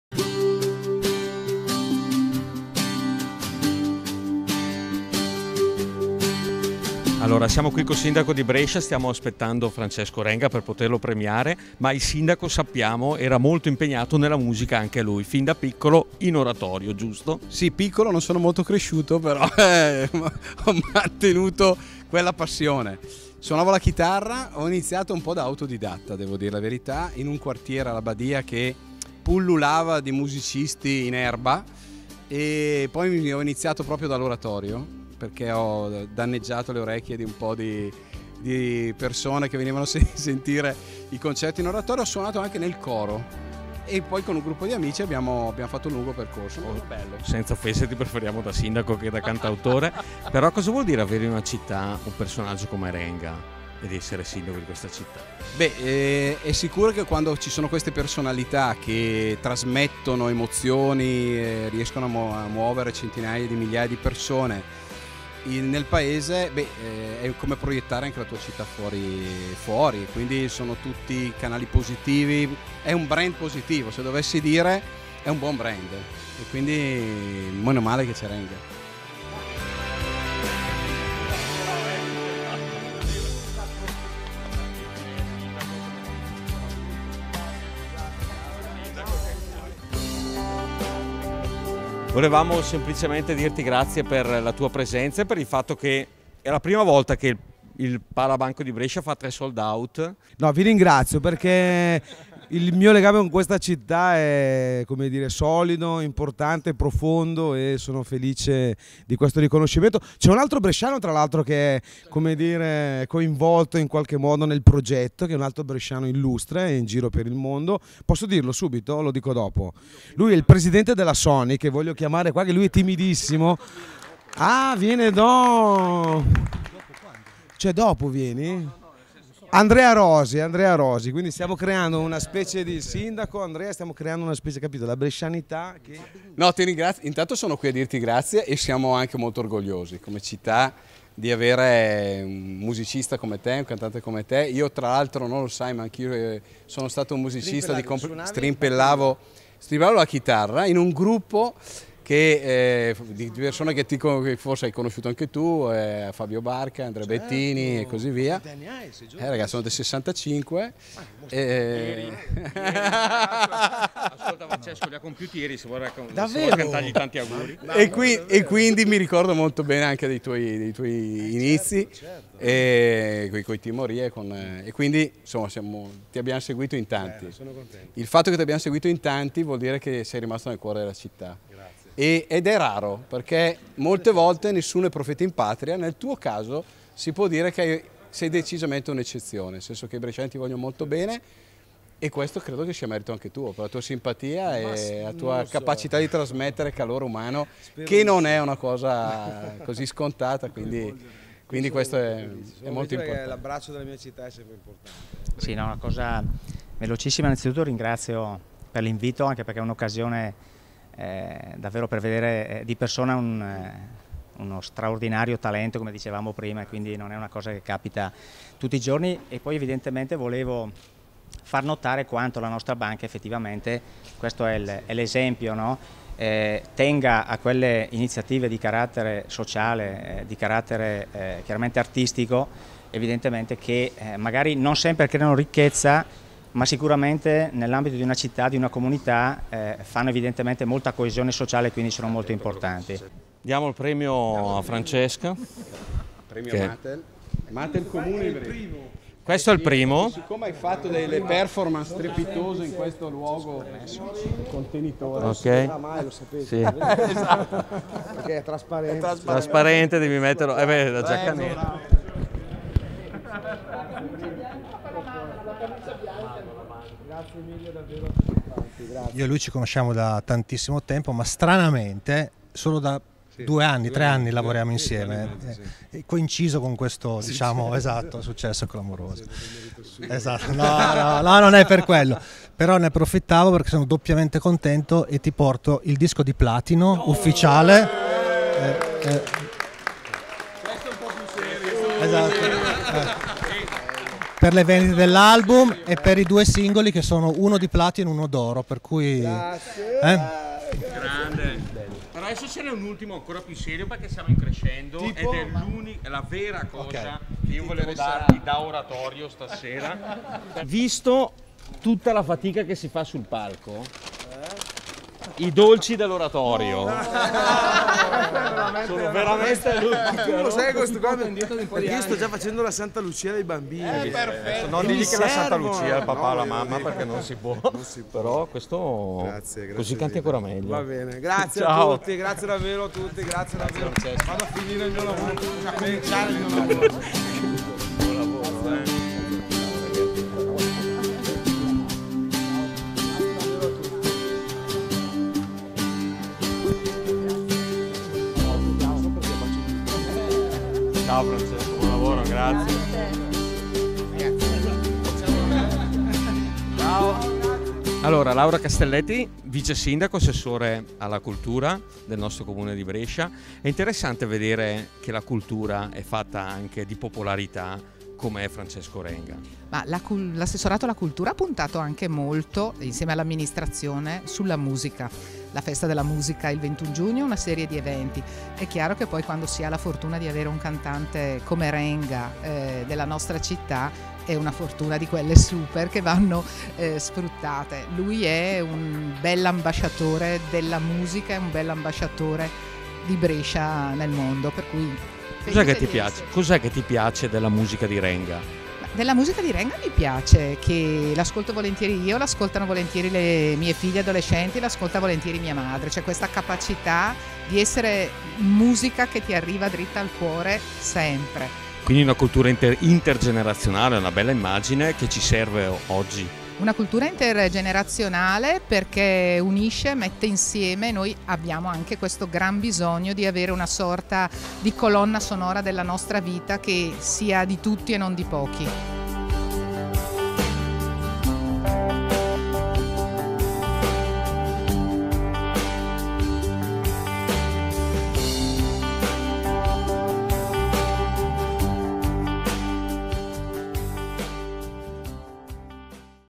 Francesco Renga riceve il premio Teatro della Città 2014 e il Disco di Platino
Giovedì 27 novembre Francesco Renga ha ricevuto il Premio Teatro della Città 2014 e il Disco di Platino nel foyer del Pala Banco di Brescia. Presenti: Giuseppe Benedetti Amministratore Delegato Matel, Emilio Del Bono Sindaco di Brescia, Laura Castelletti Vice Sindaco di Brescia, don Marco Mori Presidente COB, Maurizio Veggio Direttore Territoriale del Banco di Brescia, Andrea Rosi Presidente Sony Music Italia. Riprese e montaggio: Silvia Cascio. Assisitente: Ludovica Chiarini. Coordinamento: Simone Agnetti.